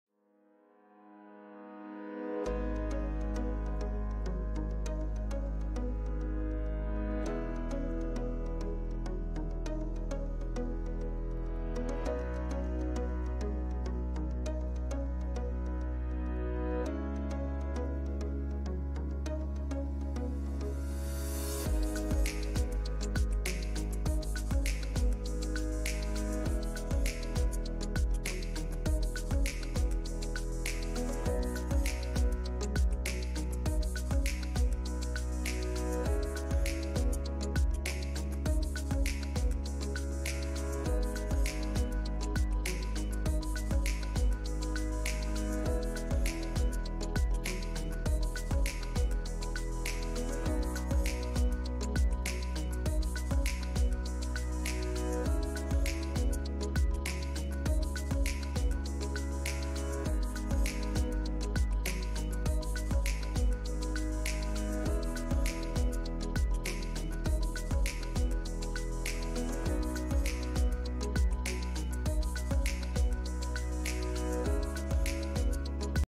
Tannin Lignin (KWC-4460E) Test Kit Instructions | Color Match | Water Conditioning
In this instructional video, we will show you how to use the Tannin Lignin (KWC-4460E) Test Kit for water conditioning and purification. This color matching test kit is an effective tool to determine the presence of tannin and lignin in your water supply, which can cause discoloration and affect the taste of your drinking water.
Our step-by-step guide will walk you through the process of testing your water, interpreting the results, and providing effective solutions for water conditioning.